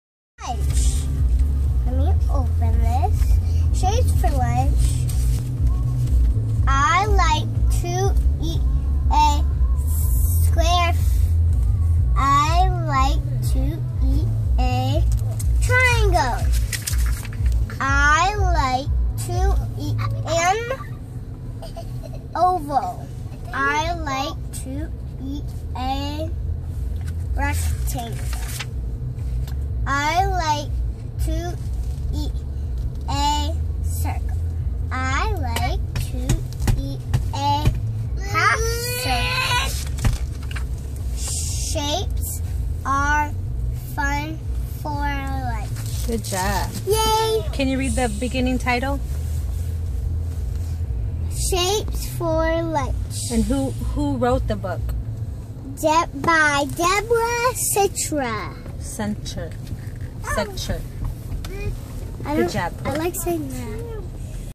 Shapes for Lunch. Aaliyah on her 3rd book .
Preschooler reading.